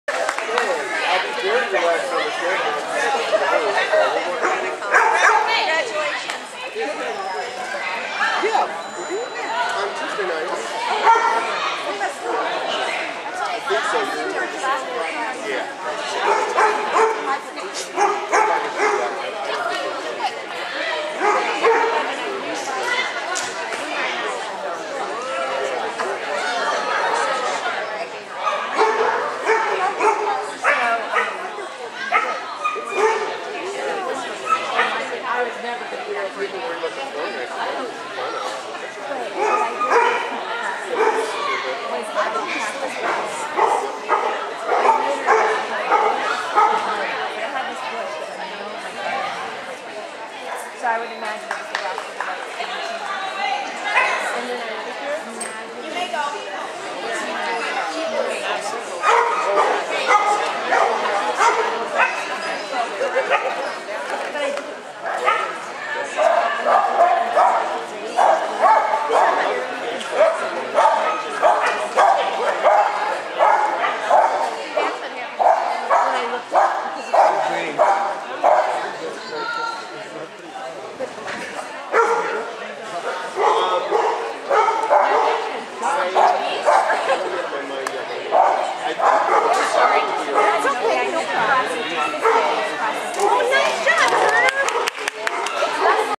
Syd-Tunner Ex JWW 3-3-12.MOD
Excellent Jumpers course at Smack Dab's trial on Sat March 3, 2012.  Sydney running first - NQ, Tunner running second - Q!